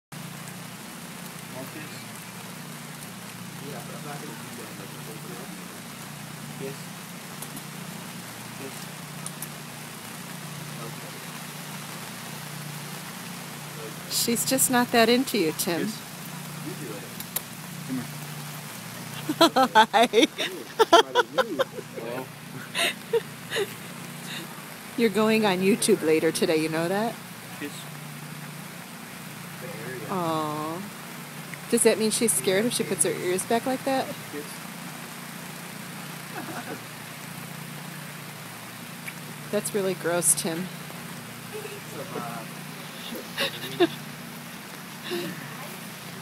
Llama Love
Tim trying to get llama to kiss him.